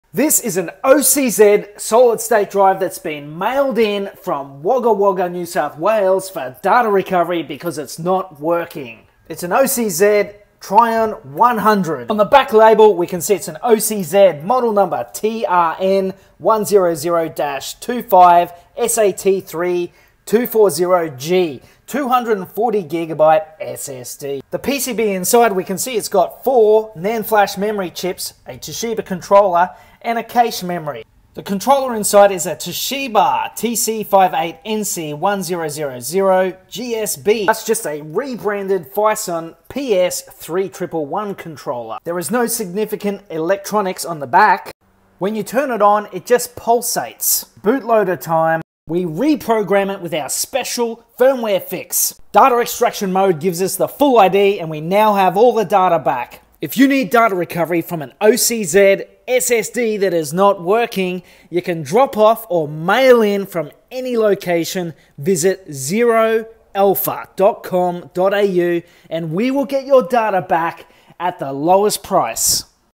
OCZ SSD Not working Data Recovery | Trion 100
This ssd was mailed in from Wagga Wagga NSW for data recovery as it stopped working. The ssd is bricked and stuck in busy mode. The 5v power input just pulses constantly. Controller: TC58NC1000GSB-00 (rebranded PS3110). Uploaded loader but utililty does not refresh ID. We we're able to get all the data back by creating a data recovery task. Model: OCZ TRN100-25SAT3-240G.

MAIL IN DATA RECOVERY: